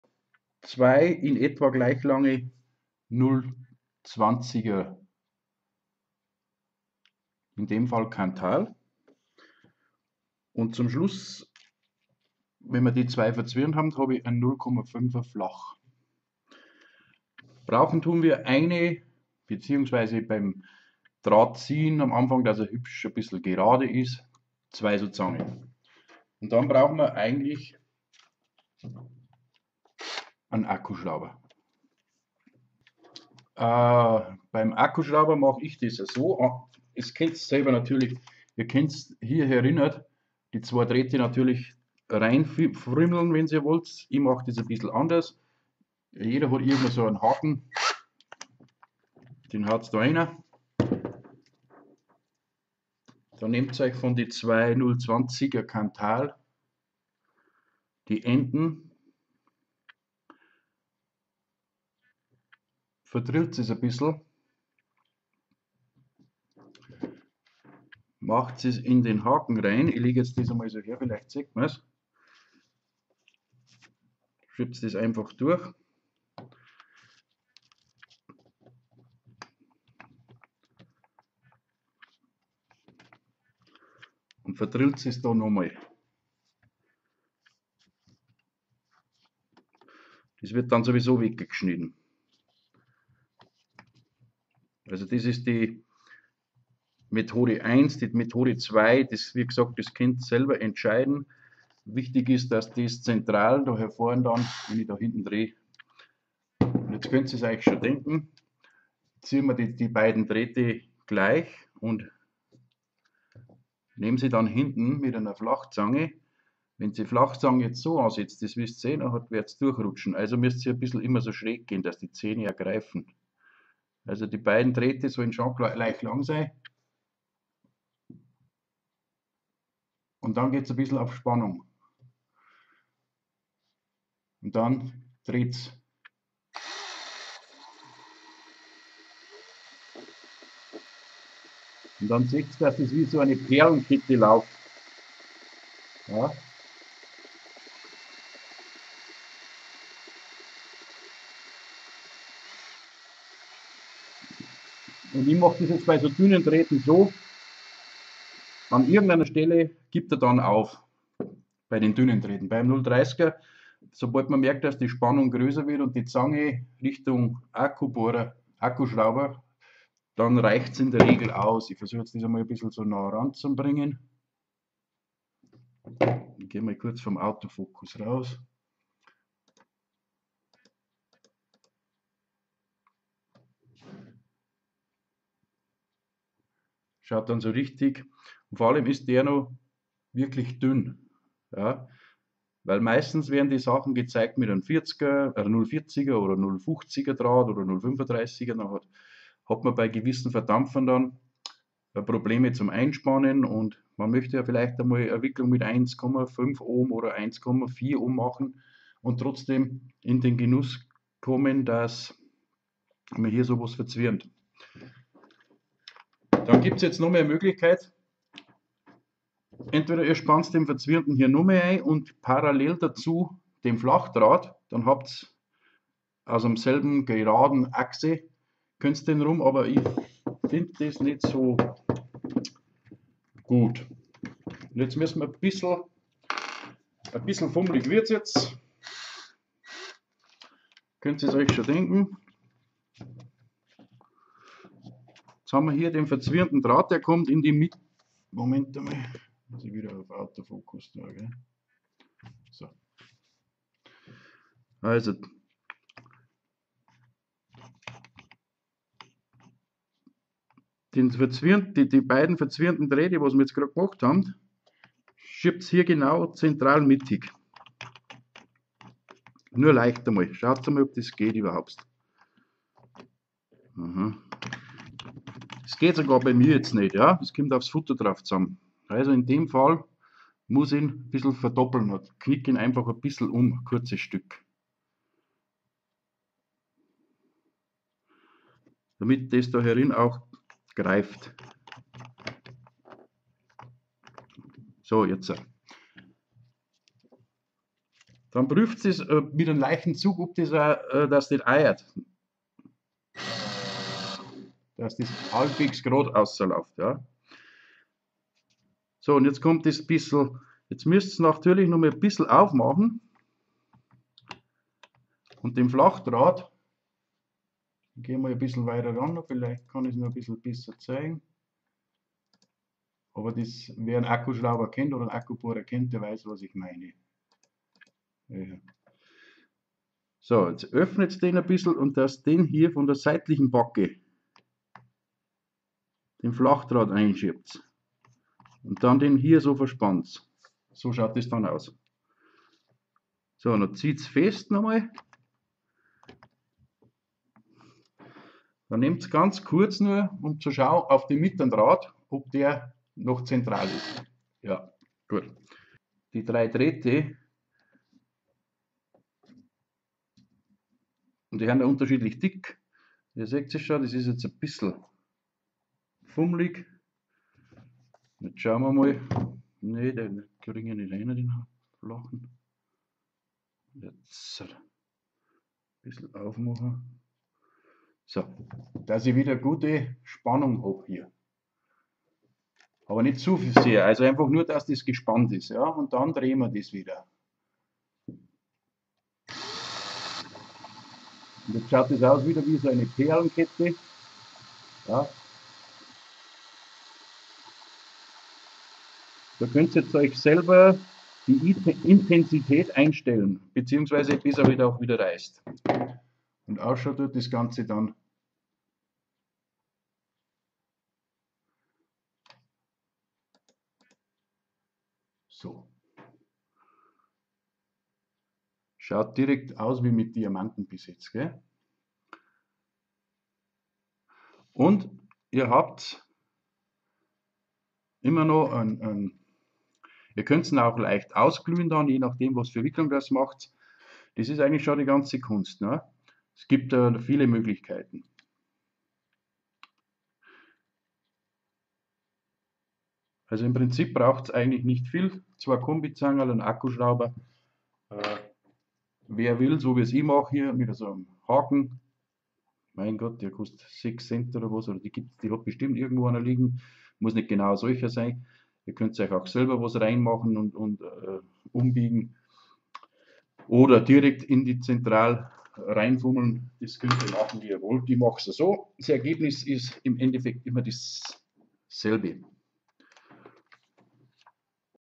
2 x 0,20 verzwirrnen und mit 0,5 flachdraht verzwi...
damit hat man einen relativ beweglichen und für normal bzw hochohmigere  wicklungen dreifach verzwirrndn draht  der nicht so dick ist und trotzdem geschmacklich top ist